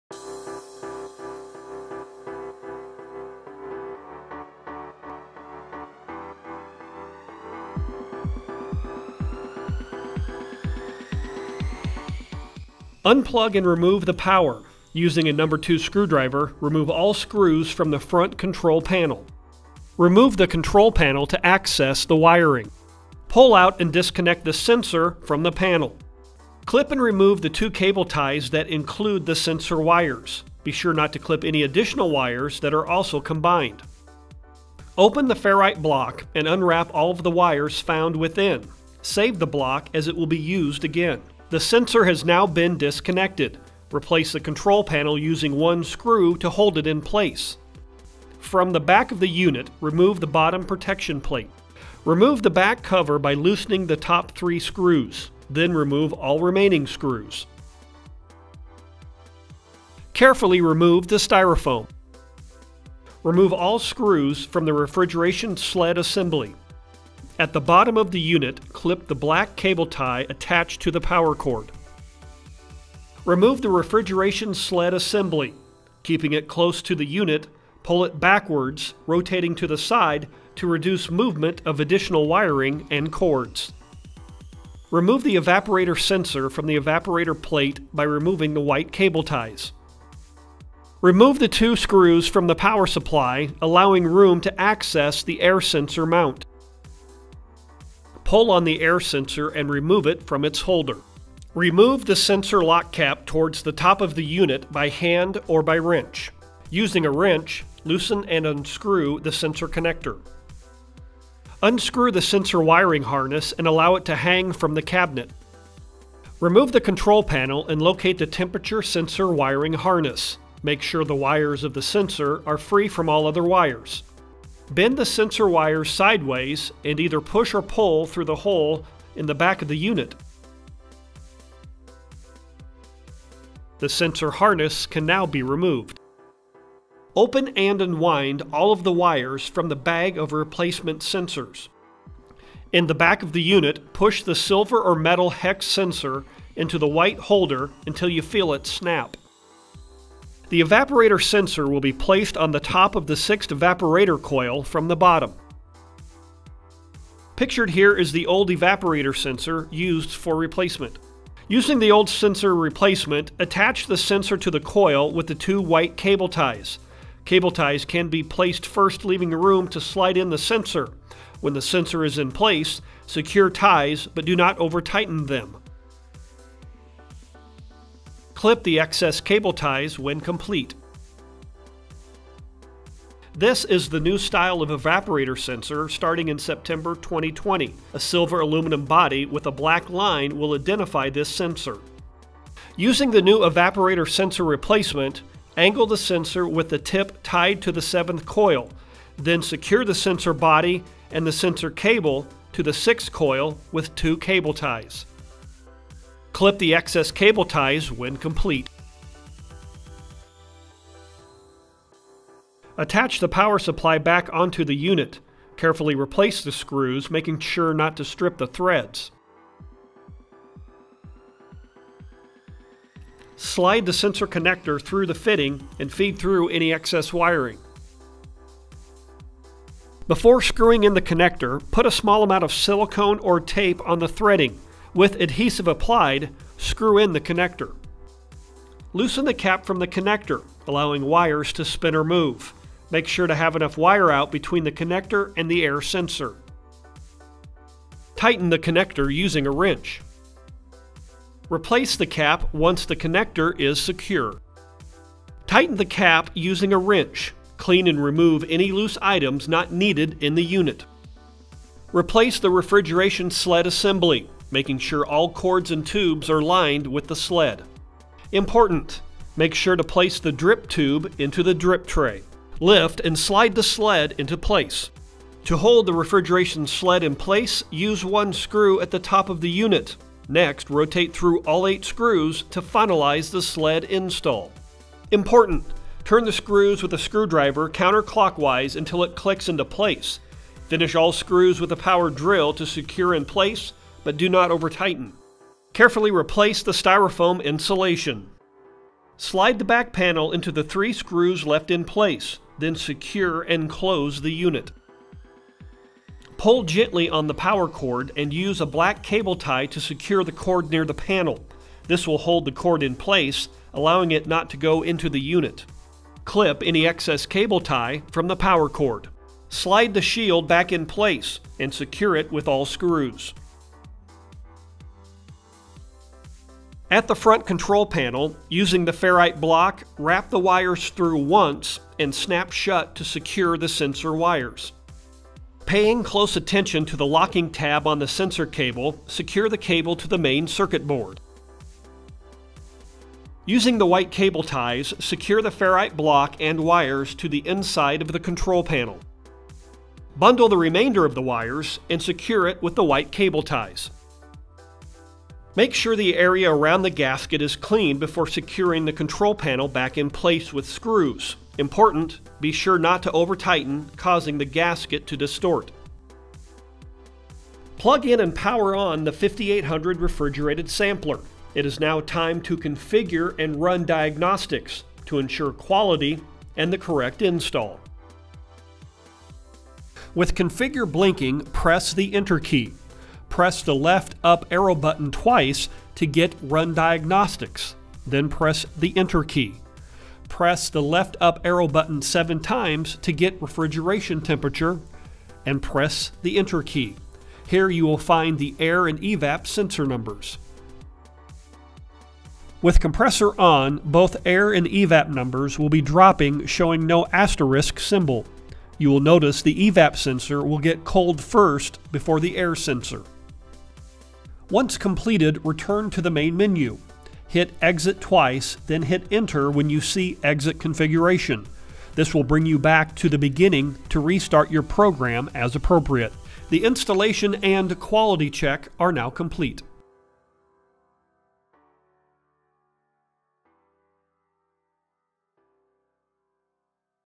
5800 Sampler - How To Replace Temperature Sensors
In this video, you will be walked through the steps on how to check to see if your temperature sensor on the 5800 Refrigerated Sampler is bad and how to replace it.